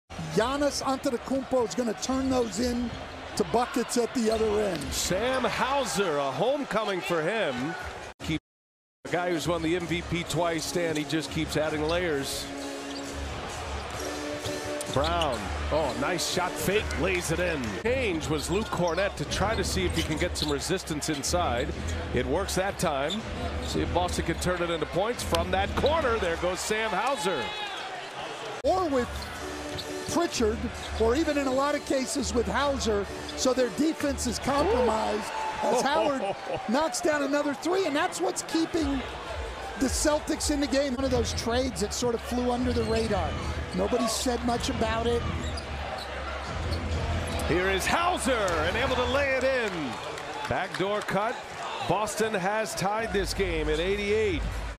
Sam Hauser 11 PTS, 4 REB, 1 AST, 3 3PM vs Bucks - Apr 7, 2022
Sam Hauser 11 PTS, 4 REB, 1 AST, 3 3PM vs Milwaukee Bucks - April 7, 2022